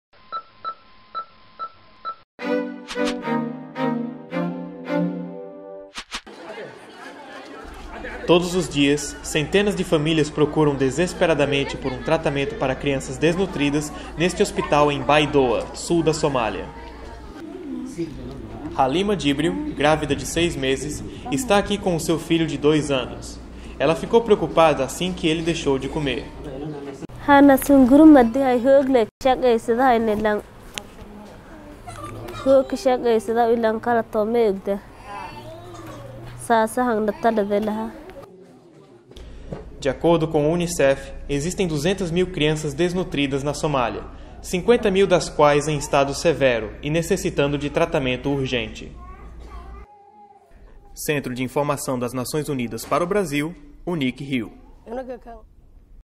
Somália: 200 mil crianças sofrem de desnutrição aguda
As áreas rurais da Somália foram o epicentro da epidemia de fome em 2011. Agora, devido a chuvas fracas e falta de acesso por questões de segurança, o número de crianças que sofrem de desnutrição está crescendo.

Atualmente, 200 mil crianças sofrem de desnutrição aguda na Somália, incluindo 50 mil que estão gravemente desnutridas e requerem tratamento imediato. Assista nesta reportagem da TV ONU a ação do UNICEF e saiba mais